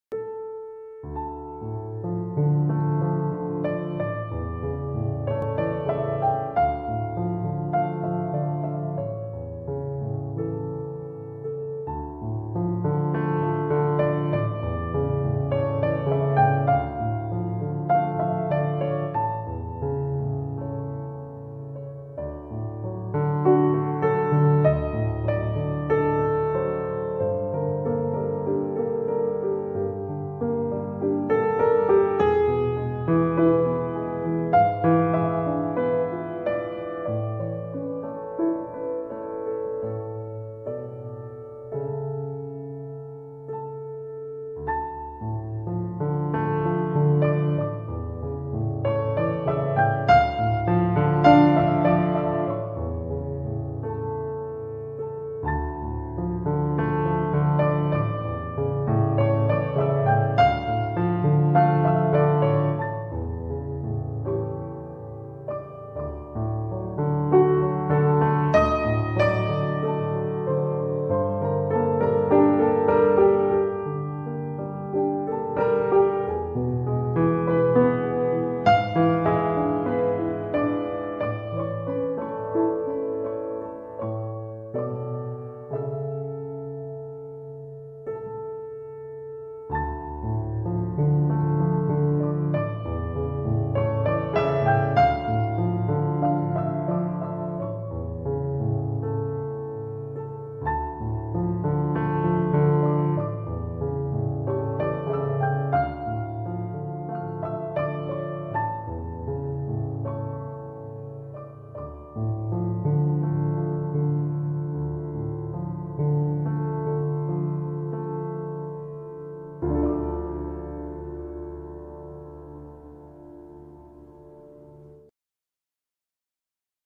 background piano - Chopin Style
some classical background piano music in Chopin style. listen, relax and enjoy.